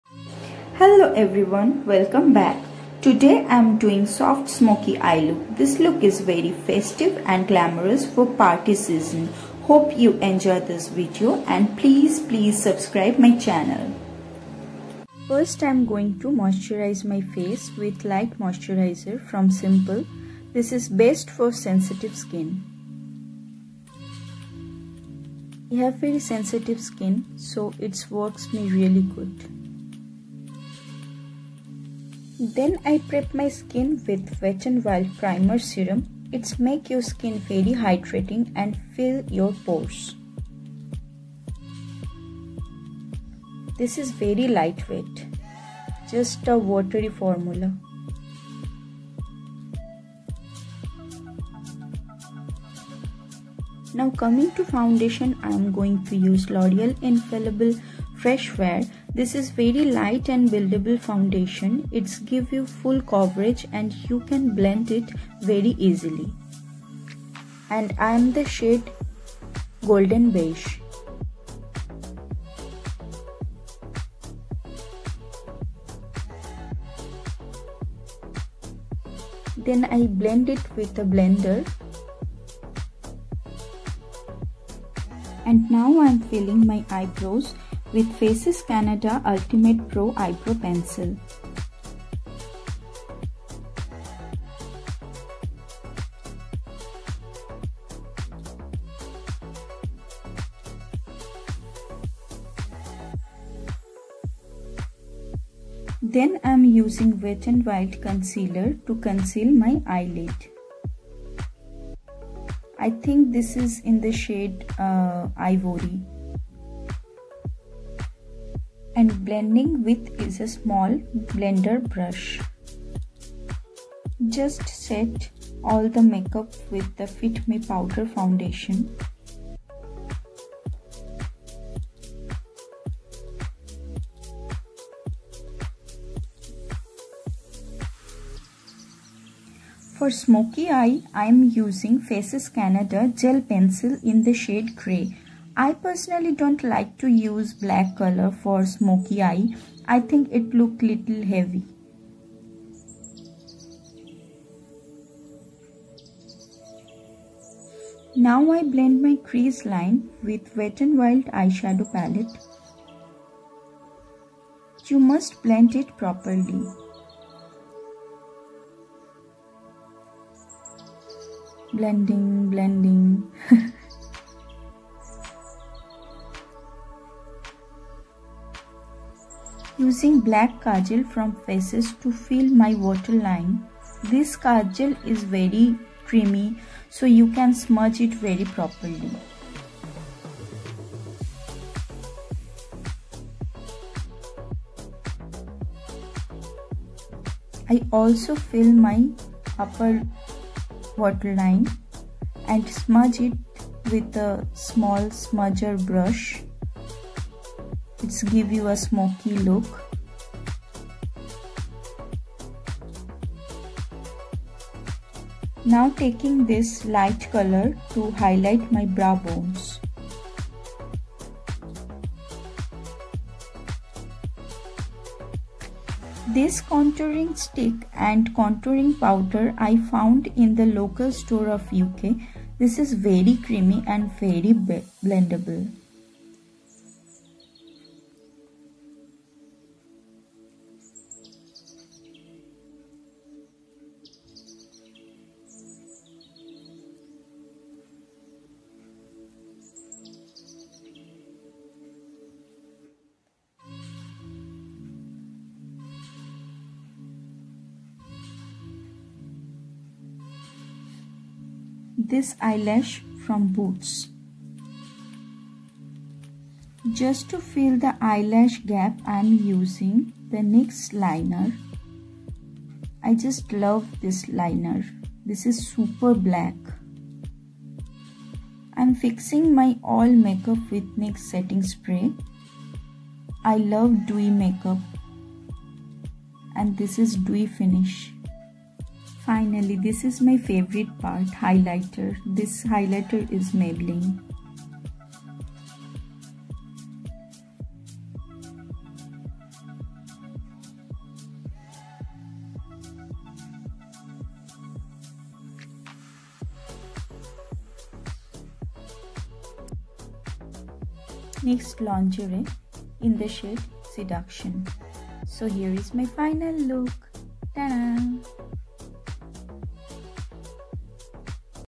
soft smokey eye look || makeup tutorial
soft smokey eye makeup tutorial

easy and simple for party look🙂